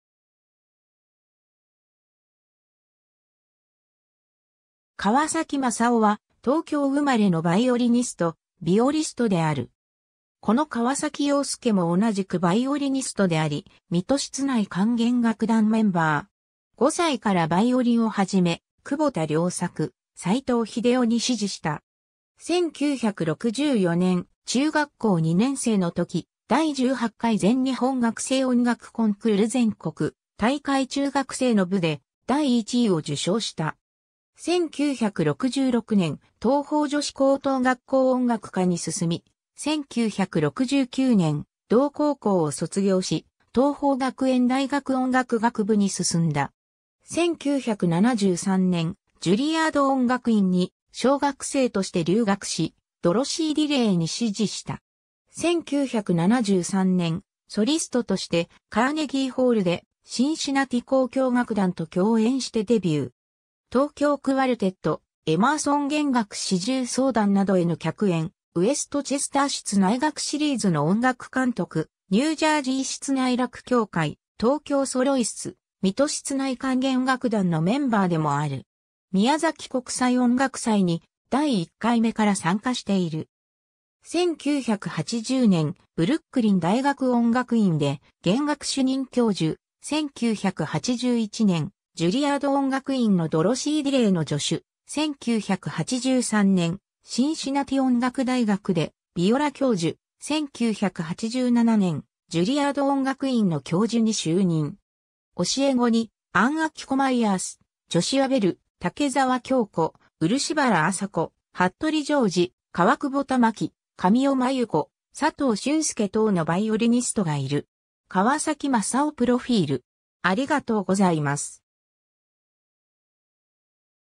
川崎雅夫
川崎 雅夫（かわさき まさお、Masao Kawasaki、1951年1月7日 - ）は、東京生まれのヴァイオリニスト、ヴィオリストである。
子の川崎洋介も同じくヴァイオリニストであり、水戸室内管弦楽団メンバー。
 5歳からヴァイオリンを始め、久保田良作、斎藤秀雄に師事した。
1964年、中学校2年生の時、第18回全日本学生音楽コンクール全国大会中学生の部で第1位を受賞した。
1966年、桐朋女子高等学校音楽科に進み、1969年、同高校を卒業し、桐朋学園大学音楽学部に進んだ。
1973年、ジュリアード音楽院に奨学生として留学し、ドロシー・ディレイに師事した。
 1973年、ソリストとしてカーネギーホールでシンシナティ交響楽団と共演してデビュー。
東京クヮルテット、エマーソン弦楽四重奏団などへの客演、ウエストチェスター室内楽シリーズの音楽監督、ニュージャージー室内楽協会、TOKYOソロイスツ、水戸室内管弦楽団のメンバーでもある。
宮崎国際音楽祭に、第一回目から参加している。
 1980年、ブルックリン大学音楽院で弦楽主任教授、1981年、ジュリアード音楽院のドロシー・ディレイの助手、1983年、シンシナティ音楽大学でヴィオラ教授、1987年、ジュリアード音楽院の教授に就任。
教え子に、アン・アキコ・マイヤース、ジョシュア・ベル、竹澤恭子、漆原朝子、服部譲二、川久保賜紀、神尾真由子、佐藤俊介等のヴァイオリニストがいる。
 ^ 川崎雅夫プロフィール